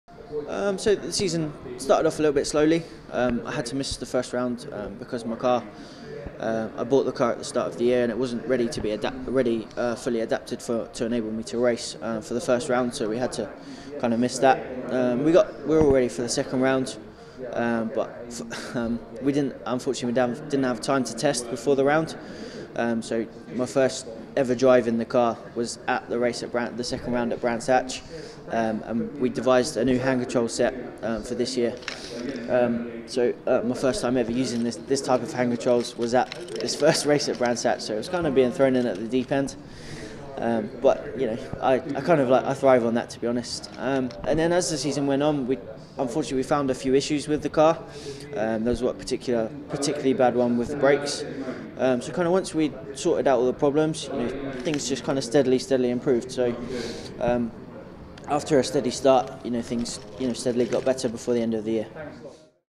Aaron Morgan on his debut season in the BMW Compact Cup
In another segment of our interview with Aaron Morgan he discusses his debut season in the BMW Compact Cup, from a struggling start to a stronger finish.